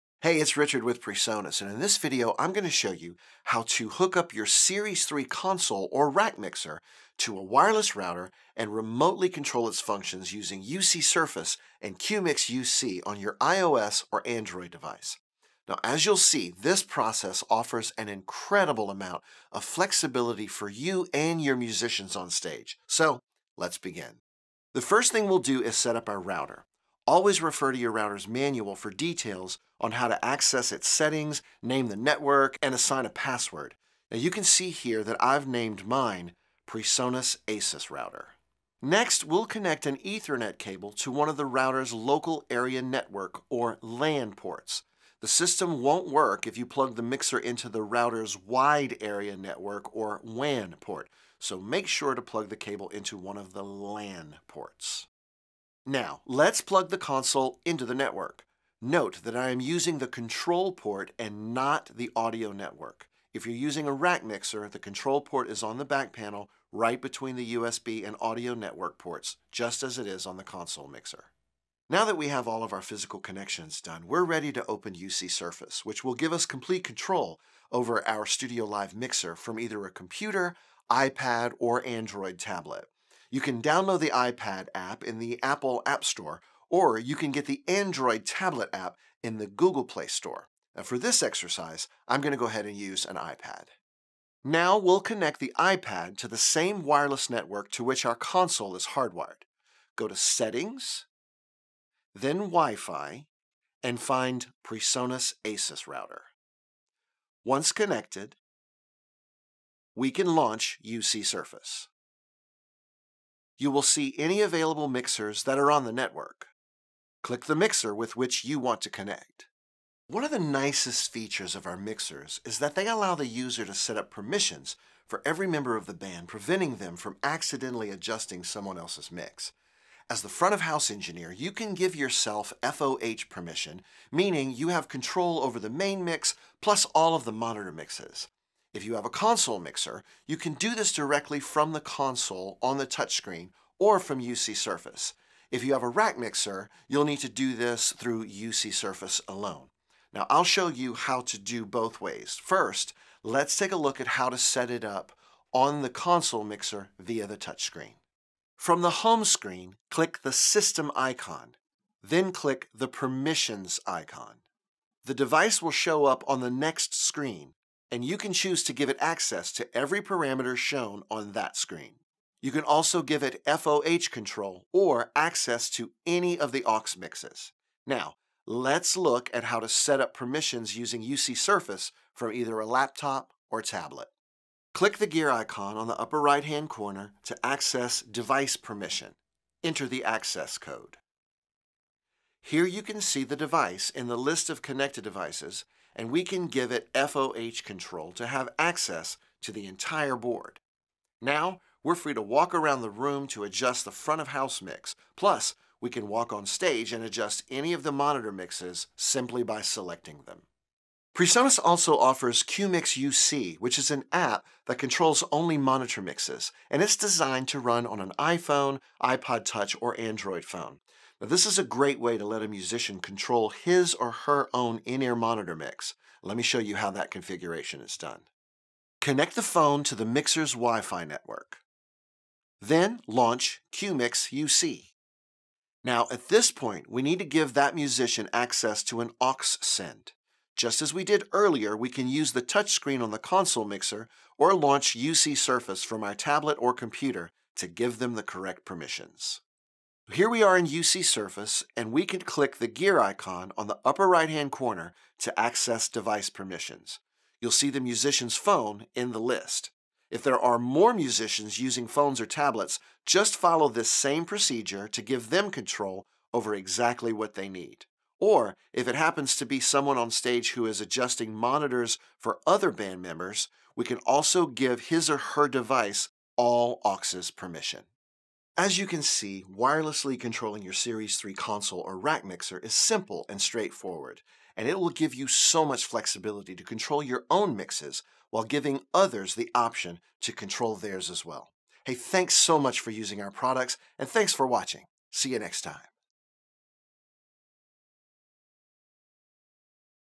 Wirelessly Controlling a PreSonus StudioLive Series III Mixer
Richard Gaspard, our product specialist, show you how to connect a wireless router, iPad, and iPhone to a StudioLive Series III console, as well as the functionality of how to use those devices with UC Surface and QMix-UC. All of the functionality shown is also available with and Android tablet, as well.